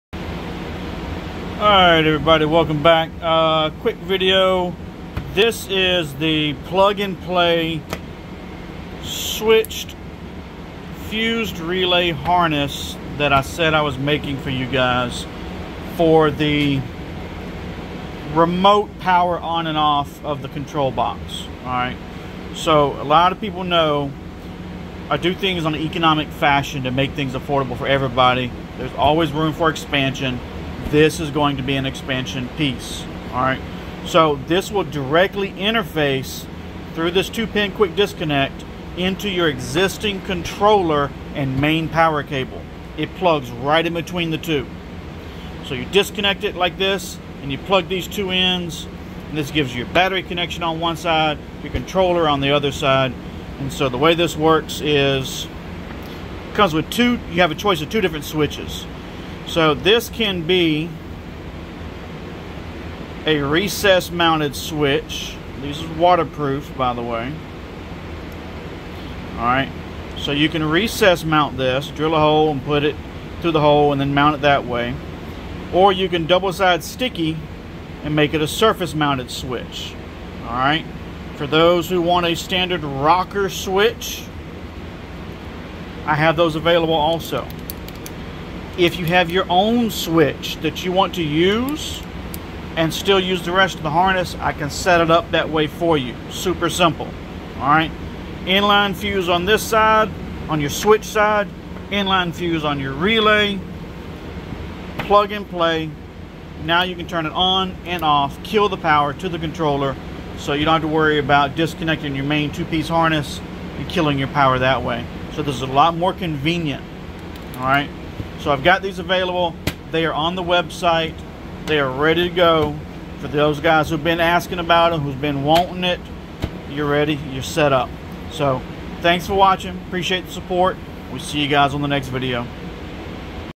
Switch and relay power harness for LED kits or others
If you are using a bluetooth controller, a lot of people think if you turn the lights off then the controller is turned off also and that is not accurate. The controller WILL still broadcast and look for something to connect to until it kills your battery UNLESS you use something like this so you can have a switch to kill power to the controller